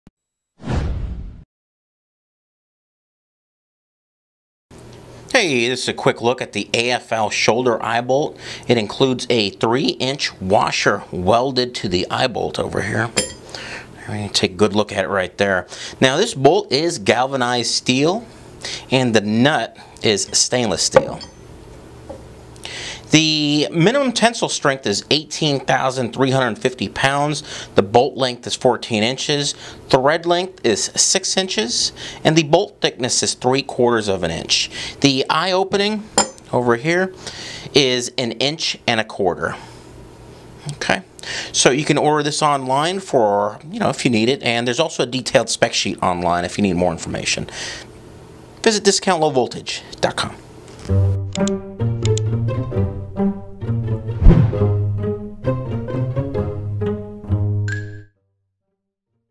Oval eye bolt for aerial cable
AFL Shoulder Eye Bolt includes 3 inch washer welded to the Eye Bolt. The bolt is galvanized steel, and the nut is stainless steel.